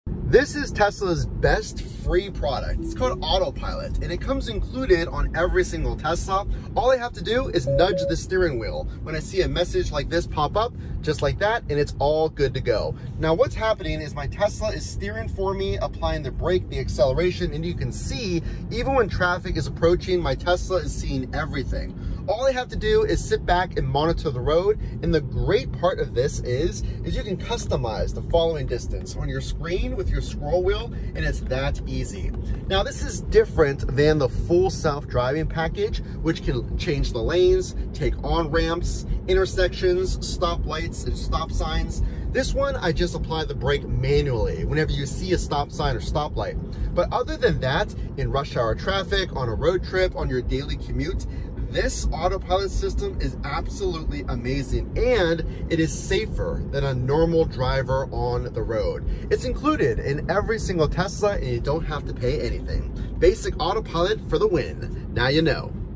Tesla Autopilot: This FREE Feature Is Amazing! 🚗
Tesla's Autopilot comes standard, offering steering, braking, and acceleration. Drivers monitor while the car handles traffic, customizable following distance. It enhances safety on commutes and road trips. Full Self-Driving requires manual braking at lights/signs.